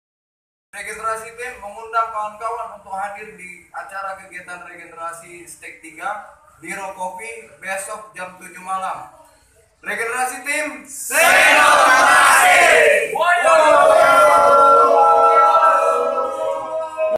REGGAENERASI # 3 'SAY NO TO RACISM'
Kami Reggaenerasi Team mengundang kawan - kawan untuk hadir di kegiatan Reggaenerasi # 3 hari jumat 2 desember 2016 jam 7 malam di Rockoffie Cafe Jln Amal no 17 AB Medan - Sunggal. Tiada kesan tanpa kehadiran kalian semua. REGGAENERASI TEAM ! SAY NO TO RACISM!